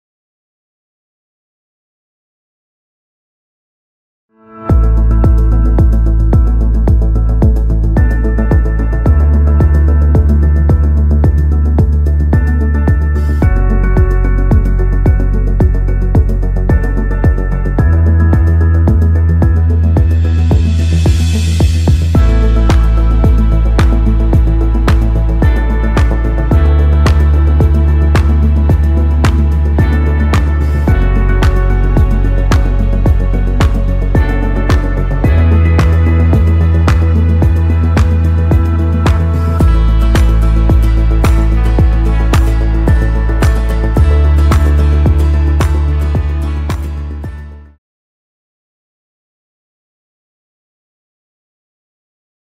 Signature Apartments Construction at Palmares Ocean Living & Golf
We are very excited to show the progress of the construction of the Signature Apartments at Palmares Ocean Living & Golf

The Signature Apartments have been designed by Pritzker Prize winning architects RCR.